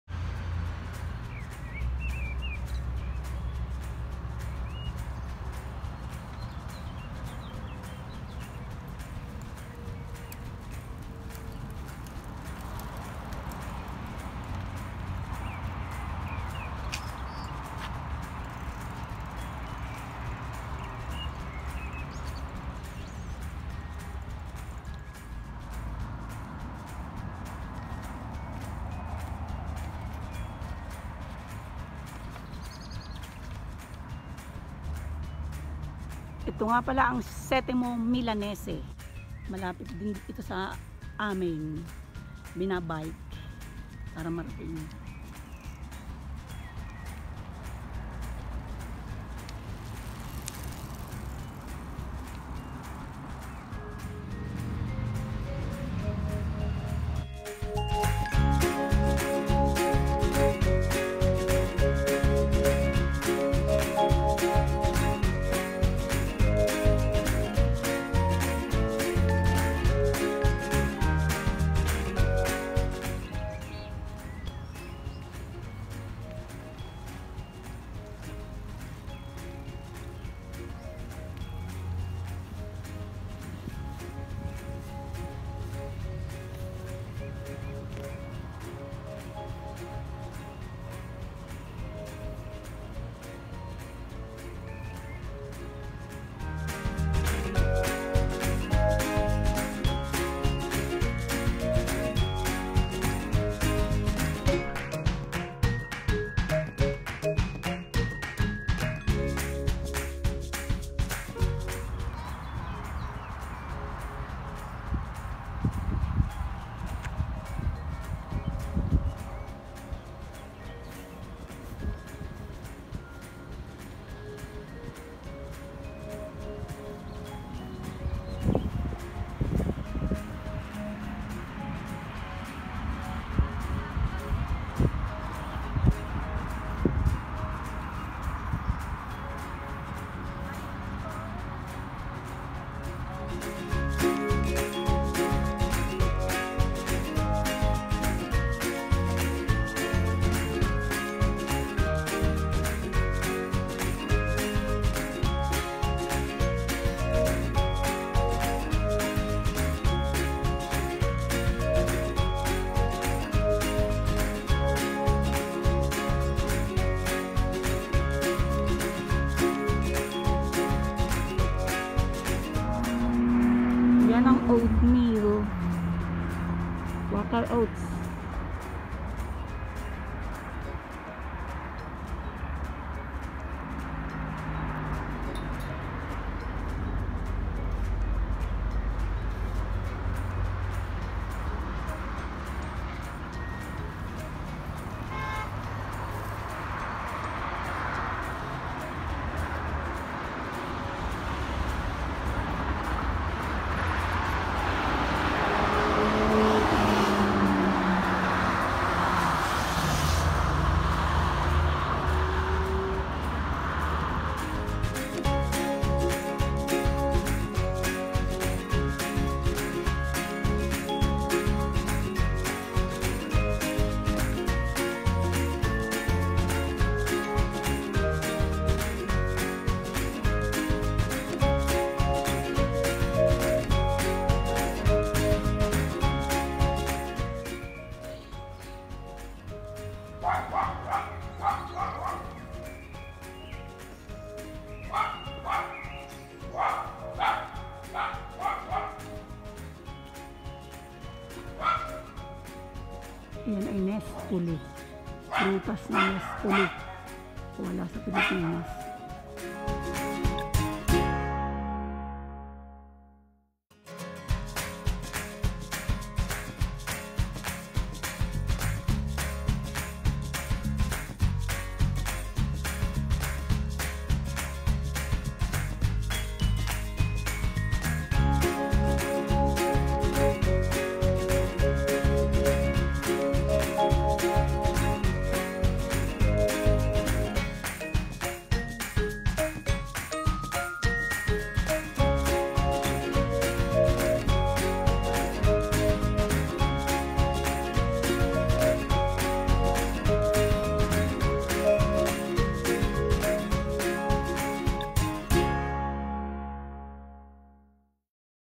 Province of Milan 🇮🇹
Settimo milanese is a province of milan 4.4 kilometers away from baggio , only 7 minutes biking 🚴‍♀️